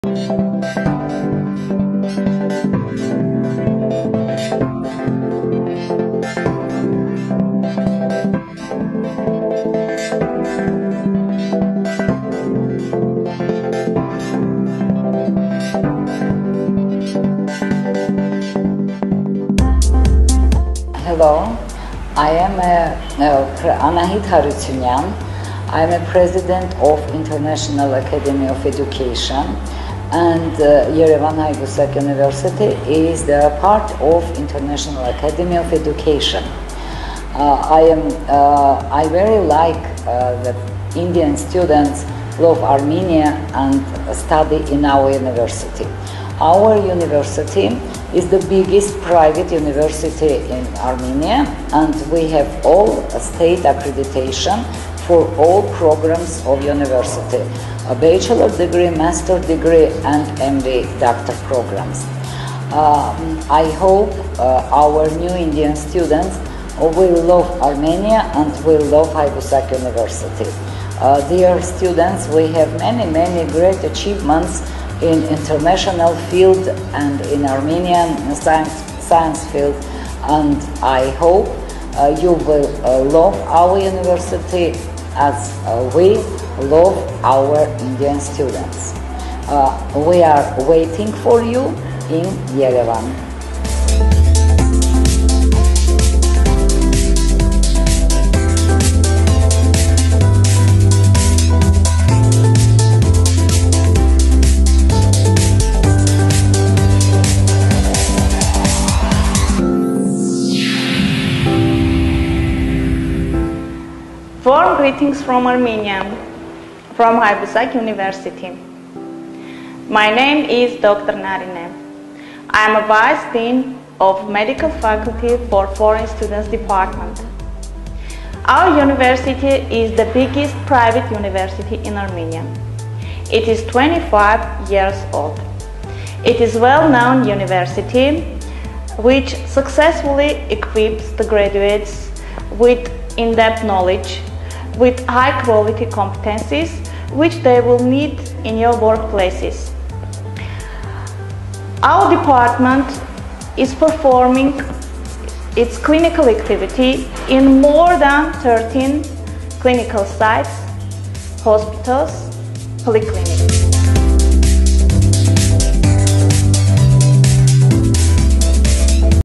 Haybusak Medical University
Study in Haybusa medical university in Armenia with 5+1 years course in English Medium having the best medical faculties and teachings stuff.